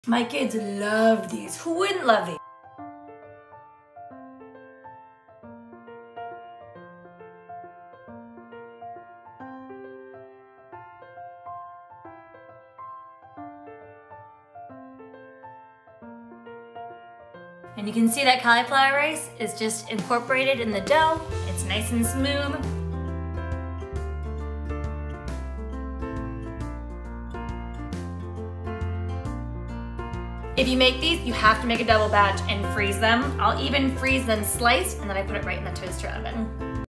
Cinnamon Raisin Cauliflower Bagels
Perfect mini bagels with CAULIFLOWER and YOGURT!

RECIPE ⬇️

From my book More Veggies Please!

Makes 8 mini bagels
2 cups all-purpose flour + more as needed
1½ teaspoons baking powder
1 teaspoon kosher salt
1 cup plain Greek yogurt (8 ounces)
1 cup cauliflower rice, fresh or thawed from frozen (3 ounces)

Topping
1 large egg

1. Preheat the oven to 350°F. Prepare a baking sheet with cooking oil spray.

2. Add all the ingredients in the food processor and process until a dough forms (it’s okay if it’s a little crumbly as long as it sticks together nicely when squeezed). Note that when your dough has formed, it’s okay if it’s a little tacky, but it shouldn’t be so sticky that it attaches to your hands. If you find this is the case, add extra flour, ¼ cup at a time, until you reach a workable consistency.

3. On a lightly floured surface, form the dough into a ball and divide the dough equally into eight pieces; I like to do this by cutting the round of dough in half, then cutting each of the halves into four pieces, like how you cut a pizza. Form the dough pieces into balls, then flatten them slightly and create the bagel holes by pushing your finger into the center of each piece of dough. Transfer the bagels to the prepared baking sheet.

4. Whisk the egg and 1 tablespoon water together in a small bowl, then brush this mixture over the tops of the bagels.

5. Bake the bagels until they are lightly golden brown—30 to 35 minutes.

Cinnamon Raisin Version: In Step 1, add 2 tablespoons brown sugar and 2 teaspoons cinnamon to the flour mixture. Once the dough has come together, place ½ cup raisins on your counter and put the dough on top of the raisins. Knead the dough a couple of times until the raisins are fully incorporated. Continue on with Step 2.